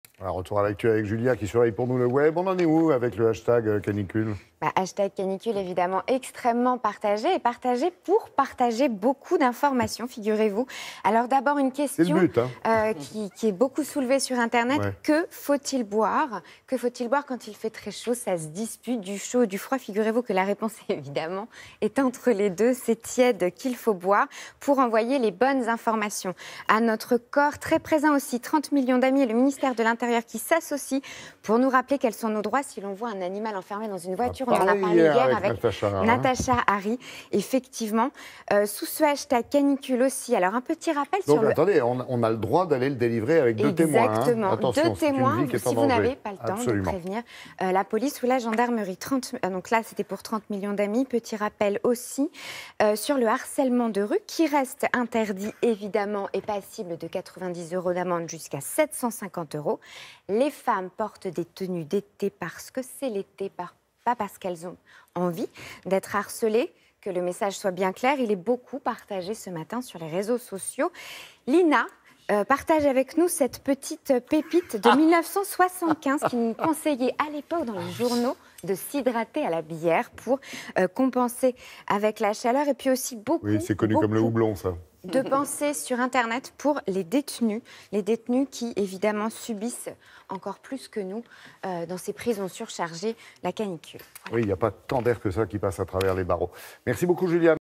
La canicule fait réagir le Web !
Chroniqueuse : Julia Livage

Depuis le début de la semaine, la canicule touche de nombreuses régions françaises. Le hashtag Canicule fait parti des tendances sur Twitter et les internautes se posent beaucoup de questions sur cet épisode météorologique.

- Faut-il boire chaud ou froid ?
- Que devons-nous faire si un animal est enfermé dans une voiture au soleil ? Quels sont nos droits ?
- Le harcèlement de rue
- La pépite de l'INA
- La canicule en prison